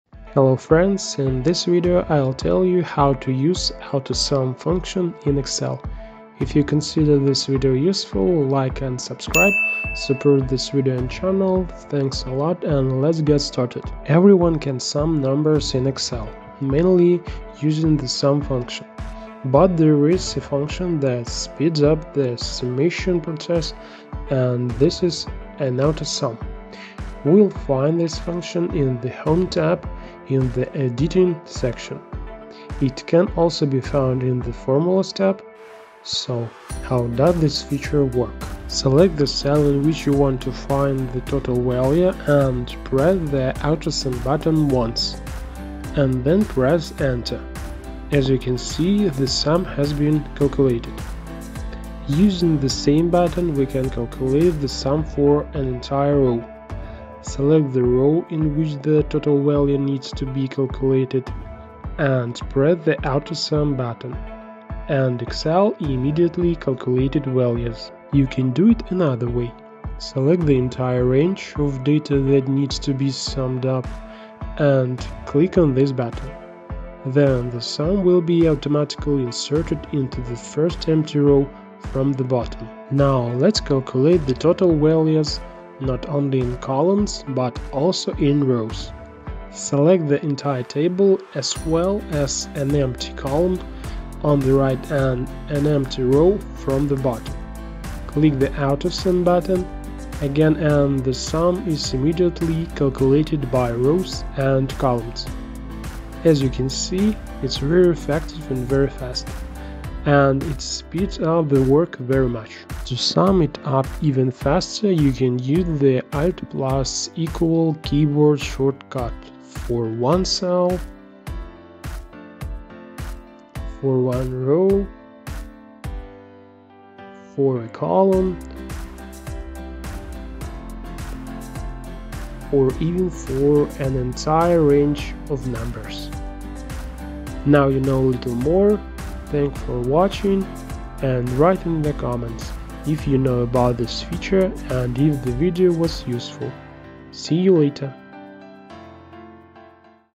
How To use AutoSum function in Excel
00:00 - Hello
00:26 - The path of the "Autosum" function
00:40 - Autosum in one cell
00:56 - Autosum in one row
01:30 - Autosum in entire table
01:52 - Keyboard shortcut